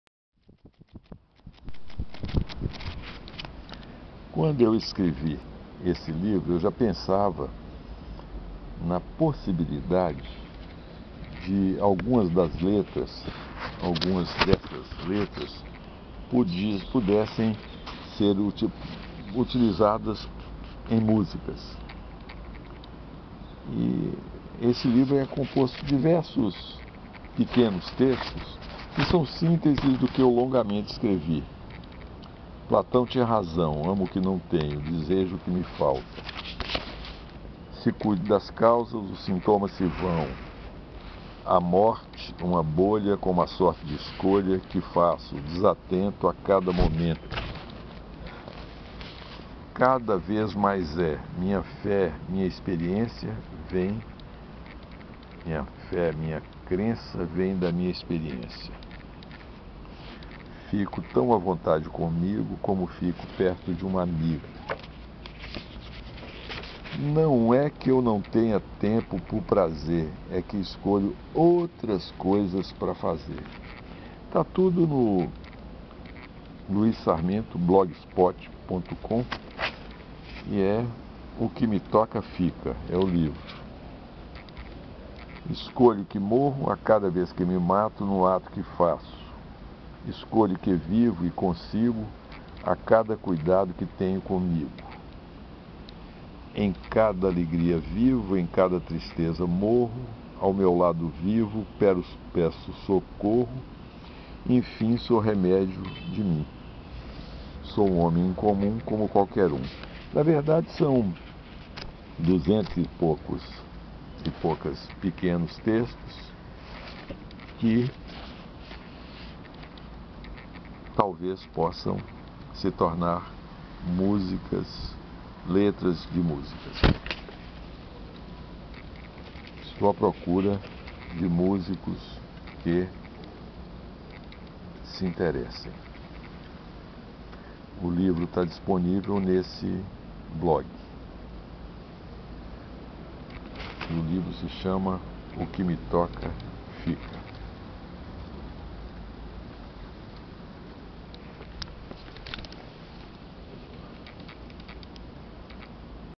546 À procura de músicas - O que me toca fica
Letras à procura de músicos,
de músicas.

Os quase-haicais que compõem
O Que Me Toca Fica
procuram músicas.

No meu desejo,
são letras
à procura de músicos.

Fantasio músicos
que, ao ler,
aqui ou ali se toquem.

E, inspirados, componham...

Arrisco.

Brasil

29 de março de 2017

29 de março de  2017